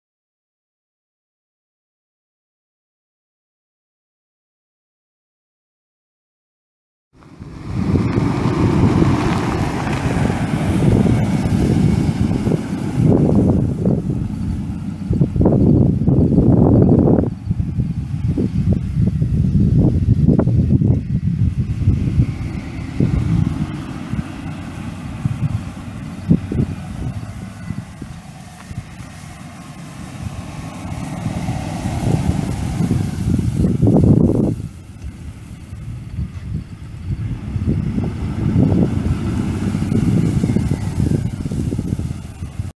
1996 Chevrolet Blazer SUV for sale | no-reserve Internet auction November 29, 2017
1996 Chevrolet Blazer SUV
62,971 miles on odometer
Miles may vary, still in use
4.3L V6 OHV 12V gas engine
Automatic transmission
Tilt
Cruise
AM/FM/cassette
Power windows, locks, and mirrors
Cloth interior
Fog lights
Front tow hooks
235/75R15 tires
Possible coolant leak
Hail damage
1GNDT13W1T2283510
It's located in Albert, KS, and we're selling it to the highest bidder regardless of price.